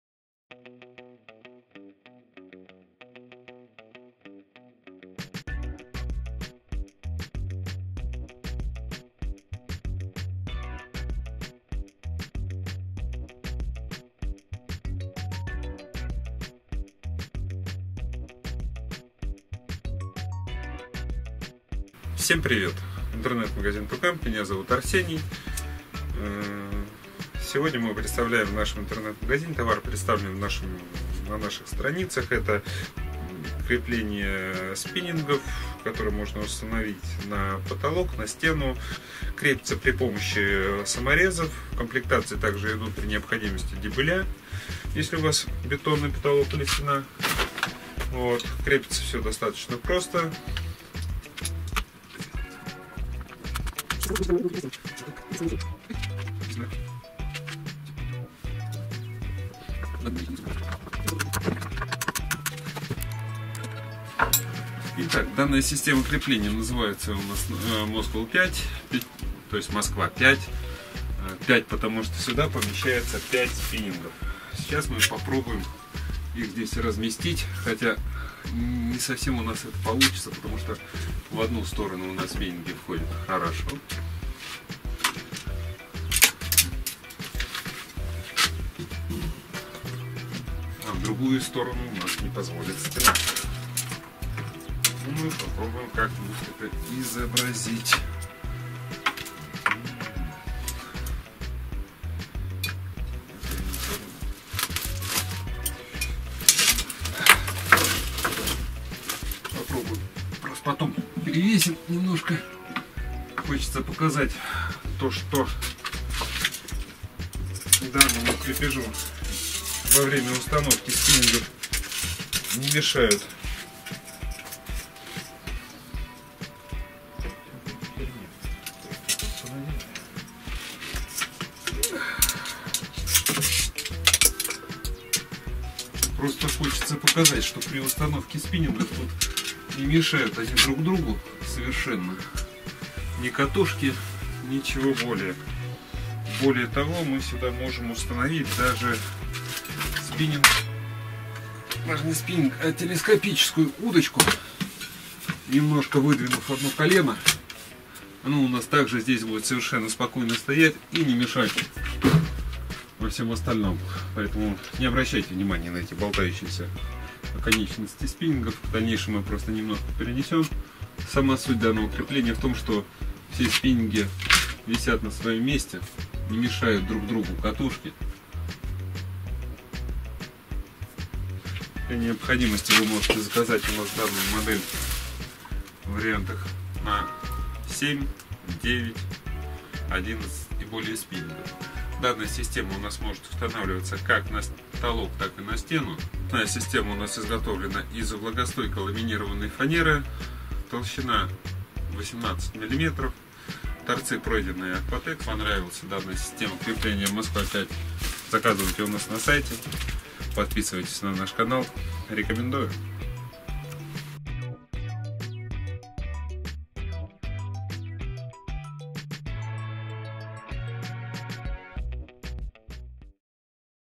Подставка для хранения удочек Moscow 5
Подставка для хранения удочек Moscow 5 выполнена из влагостойкой ламинированной фанеры, торцы пройдены пропиткой для защиты от влаги и бактерий. Данная подставка поможет вам сохранить спиннинги в целости и сохранности.